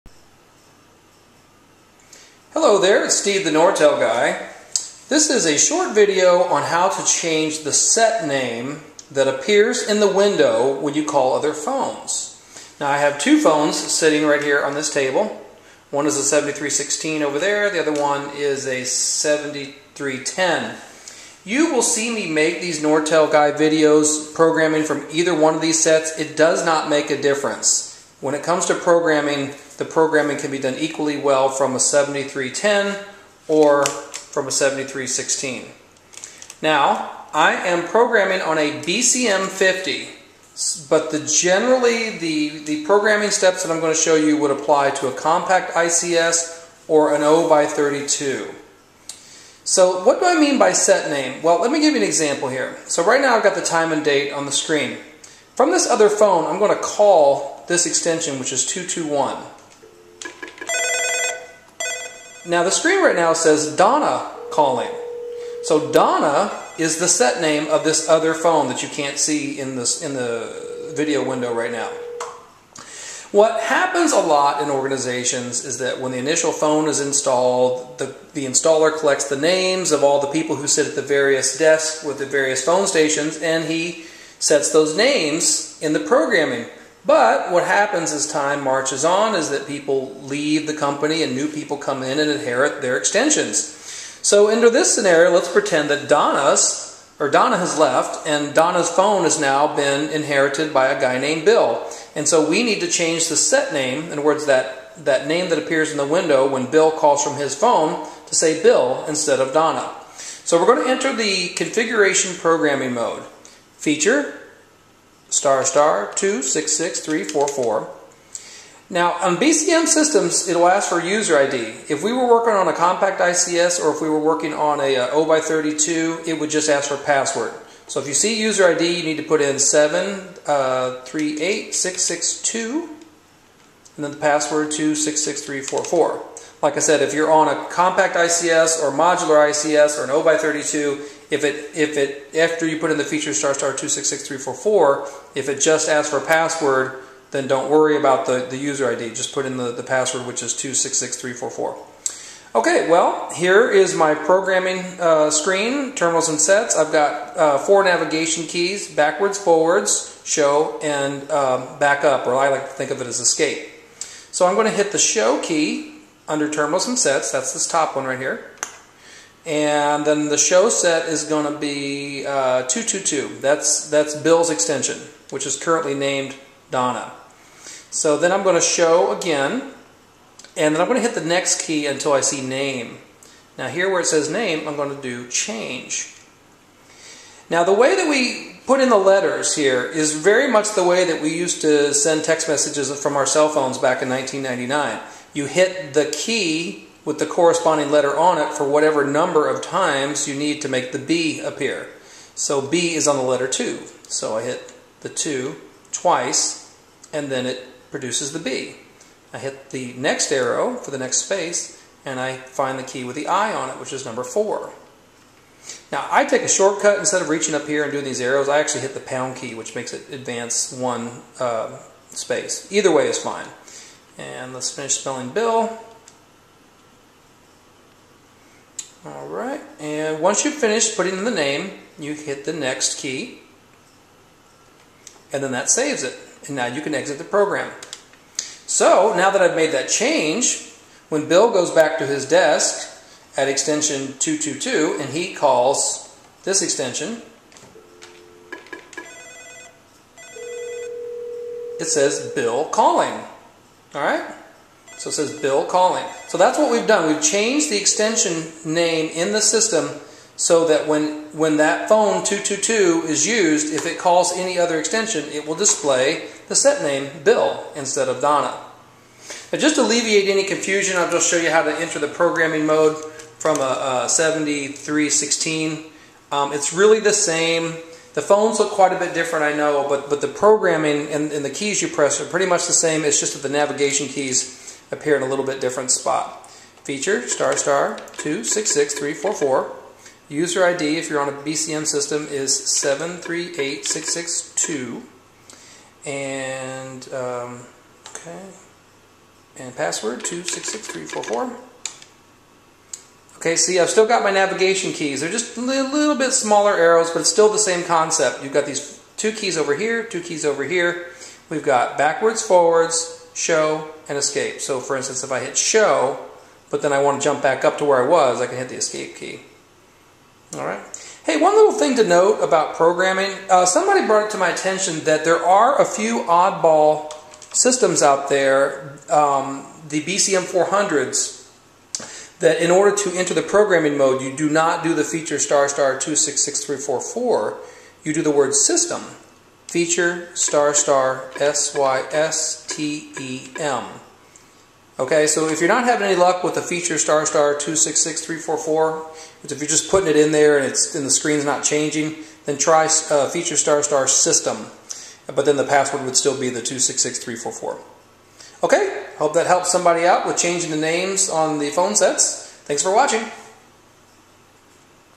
Change Name on Nortel phone
A quick and simple video on how to change the name on the Nortel Norstar phone.  This is also referred to as the "Set Name".

The name that appears on other's phone sets when a particular station is calling is called the "Set Name".  For instance  "Donna calling".  DONNA is the set name.

If your set only shows numbers i.e. "222 calling'.  That means the set names have not been programmed and you are free to do so.

Schedule a SERVICE visit for locations in Central FLORIDA